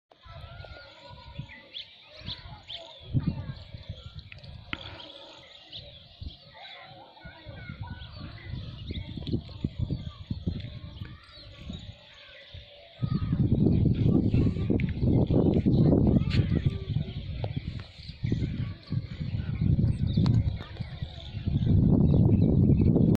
Double Rainbow🌈 Beautiful Nature| Amazing Nature showing its Beauty
Full Rainbow seen on a Rainy Day
When i saw,it was really amazing and beautiful